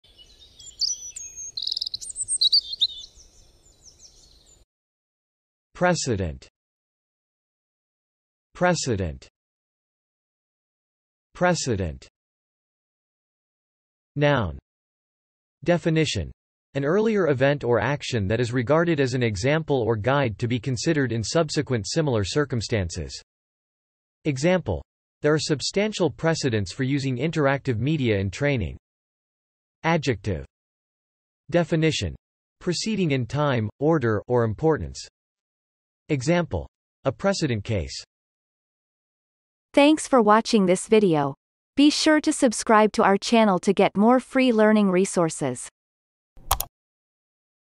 How to pronounce precedent - Vocab Today
The pronunciation of the word precedent in Amercian accent is demonstrated in this video. If the word is from another language, such as brand name, it will be pronounced in its original language. Based on the word frequency, this is of 3 stars difficulty.

The word precedent has the following meanings:
1. (noun)
Definition : an earlier event or action that is regarded as an example or guide to be considered in subsequent similar circumstances.
Example : there are substantial precedents for using interactive media in training
2. (adjective)
Definition : preceding in time, order, or importance.
Example : a precedent case